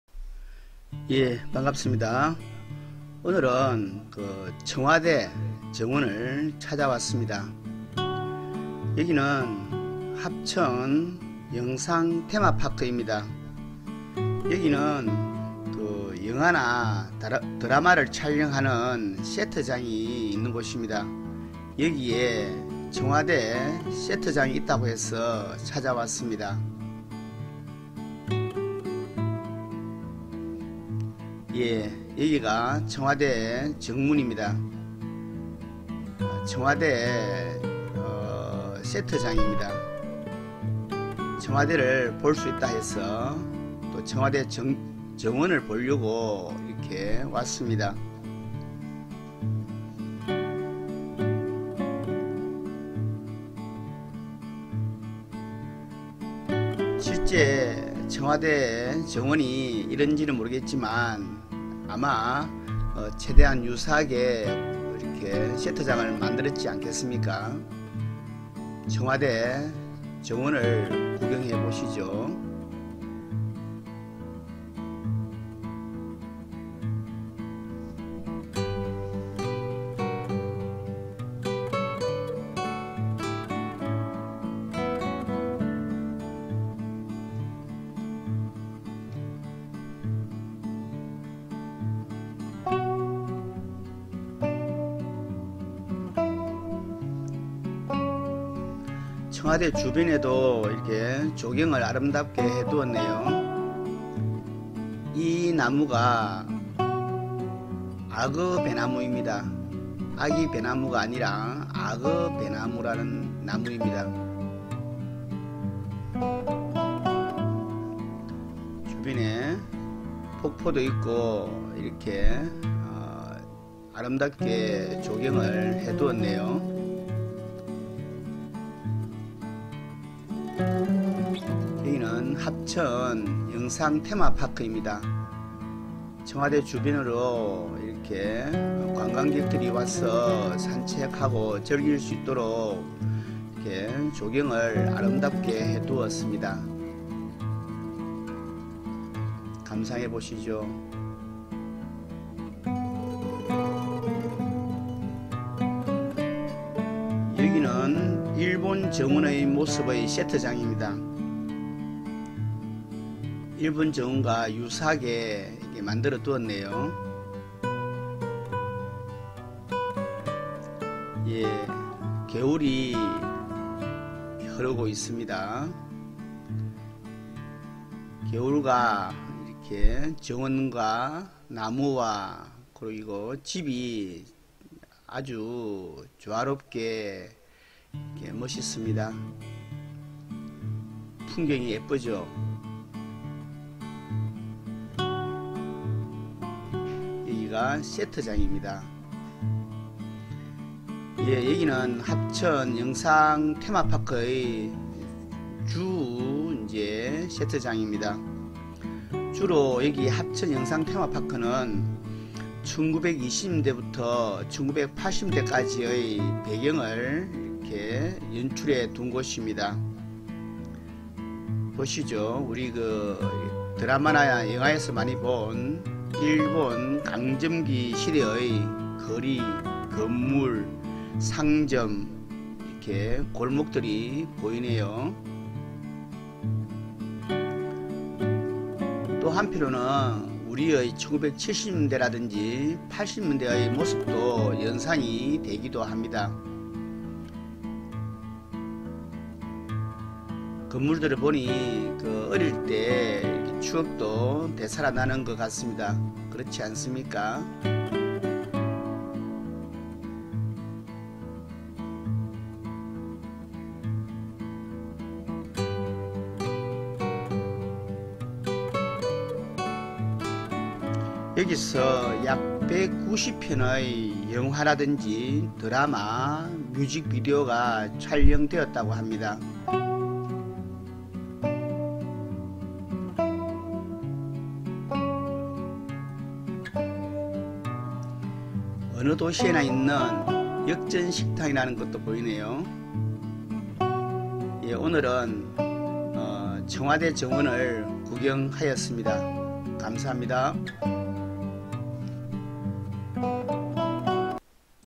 청와대 정원에 다녀왔습니다, 청와대 영화 세트장, 합천영상테마파크, 합천 여행, 정원 여행
[한국의정원]#27, 청와대 정원,
- 청와대 영화 세트장,  합천영상테마파크, 합천 여행,
- 경남 합천에 있는 합천영상테마파크 내 청와대 영화 세트장입니다.